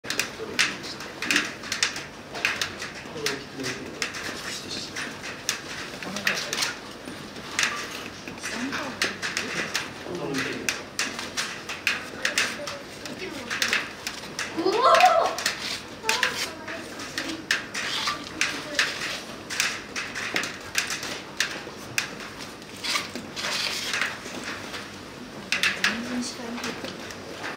口元カクカクなエルくん♪緊張緩めるとコマヌル顔♪ マヌルネコ 1/4の那須どうぶつ王国 00303
口元カクカクなエルくん♪緊張緩めるとコマヌル顔♪
マヌルネコ
1/4の那須どうぶつ王国
栃木県那須町
2020年1月4日